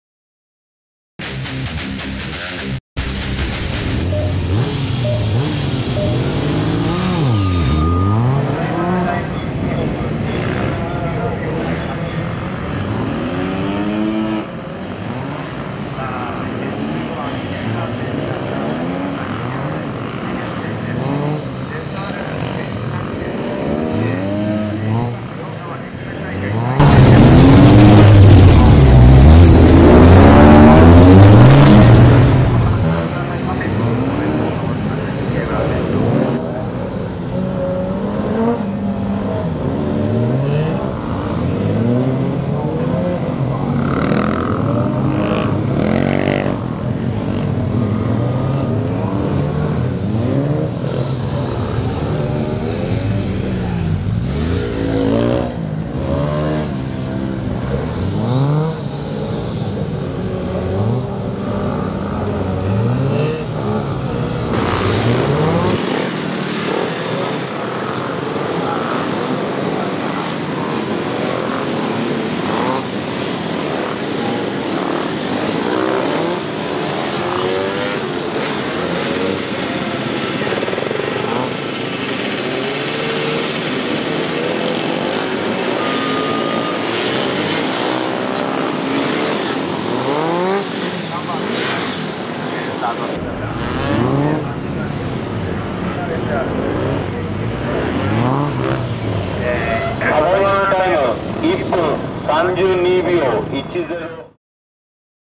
【Pick UP!】ZRX1100 池浦選手
DUNLOP・月刊オートバイカップ！ジムカーナ 2019 Round.3
JAGE認定B級の池浦選手のタイムアタックです。
重たいビッグネイキッドを軽快に走らせます。